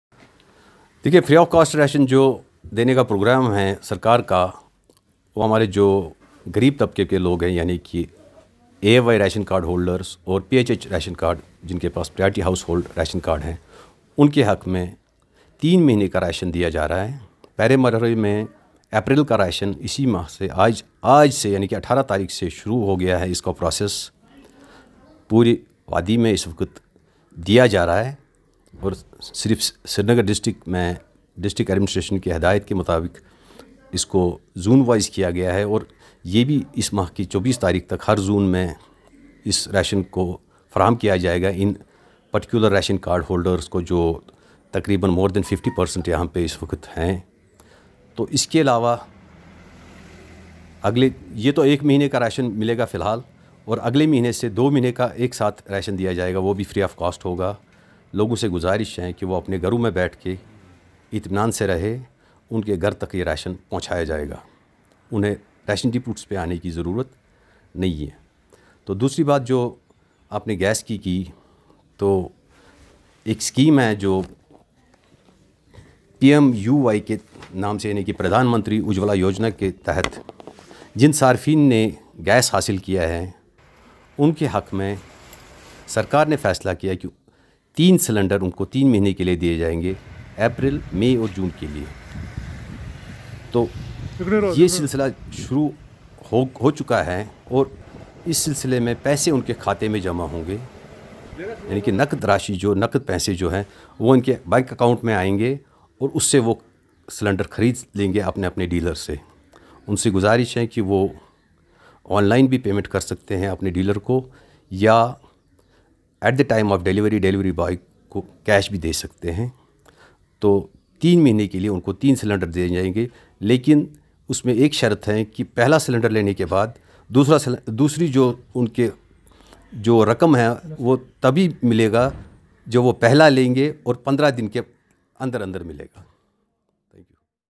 J&K govt begins distribution of ration & Gas Cylinders ..Ejac president Riyaz Ah.Sofi
J&K Govt begins distribution of free ration & Gas cylinders. Ejac & Food & supplies president Riyaz Ahmed Sofi.